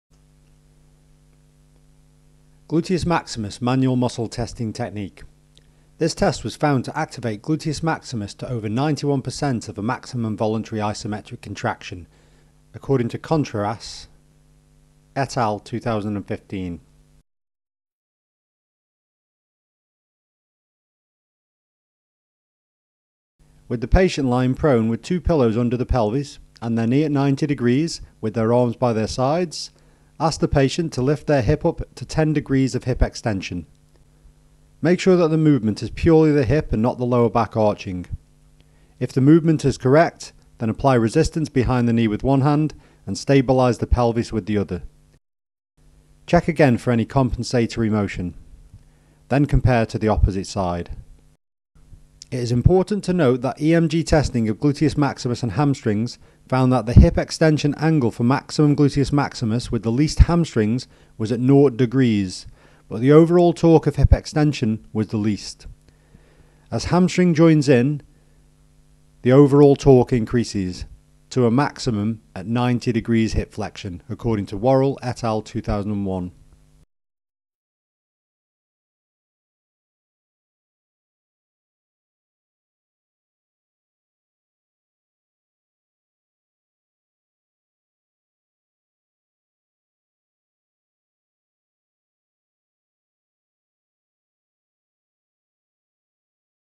Hawkes Physiotherapy E Course: Basic manual muscle testing: Gluteus Maximus
This E course is a video demonstrating how to perform a basic manual muscle testing technique for Gluteus Maximus